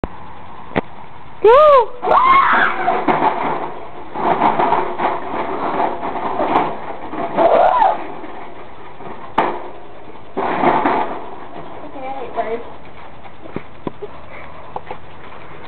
BIRDS
my friend Samantha torturing my birds. We're idiots.